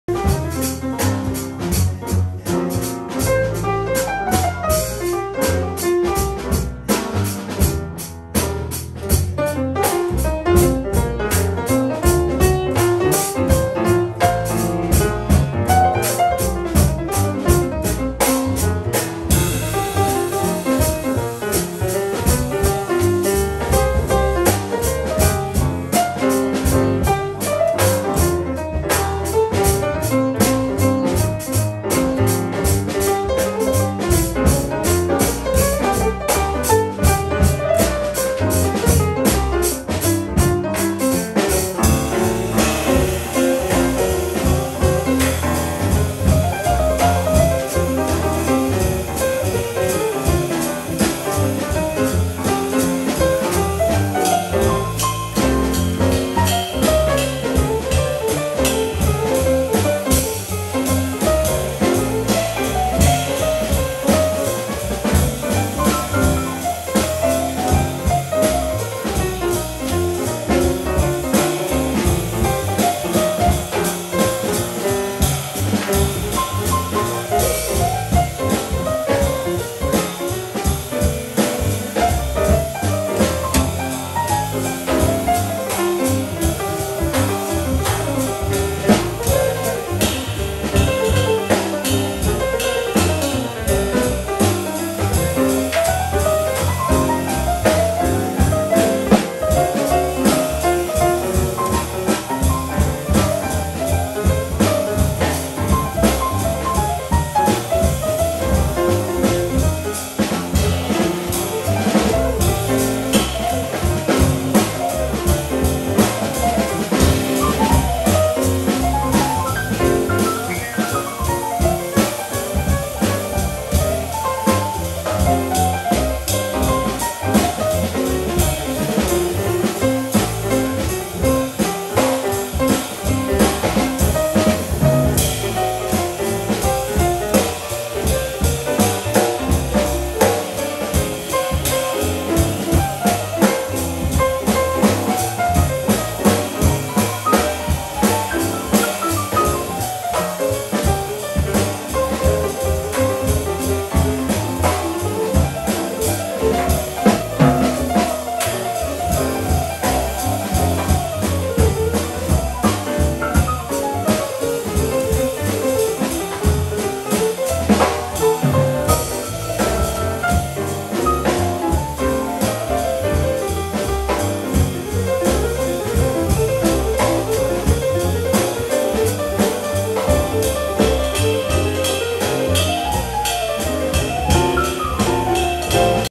ユキ・アリマサ Trio ユキアリマサ(Ｐ)・若井俊也(B)原 大力(D)
ユキ・アリマサ Trio

ユキ・アリマサ(Ｐ)・若井俊也(B)・原 大力(D)

Aug 24, 7:30 pm JST, my trio will be live-streaming our music. Please visit the link!
2021
8月24日 7:30pmよりトリオをライブ配信をします。スタンダードとオリジナルをお送りします。是非お聴きください！　配信ライブ。